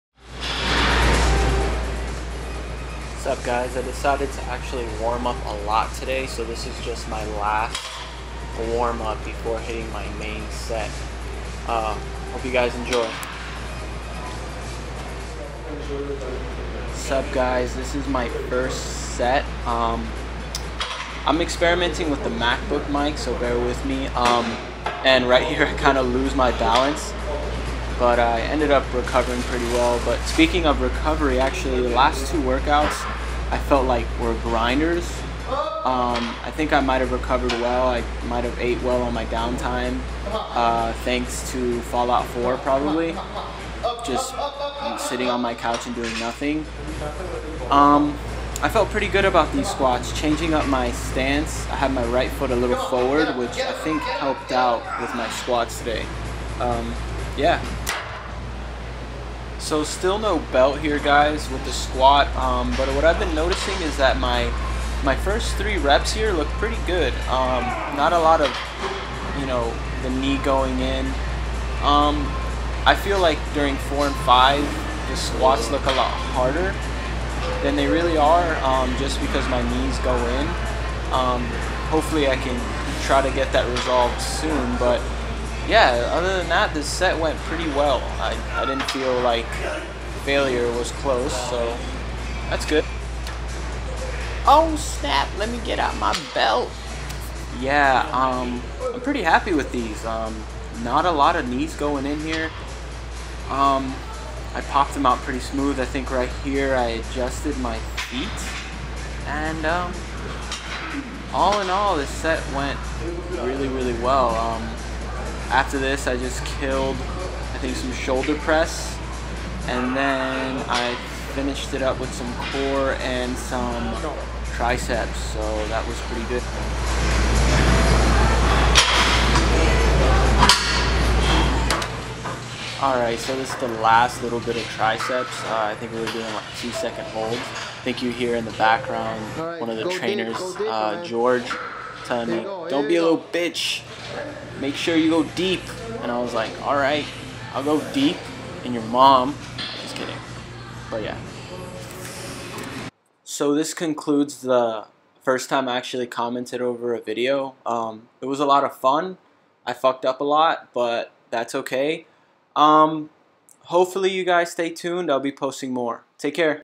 Beyond the Barbell Vlog
Eighth week using the Starting strength program, recording to improve technique.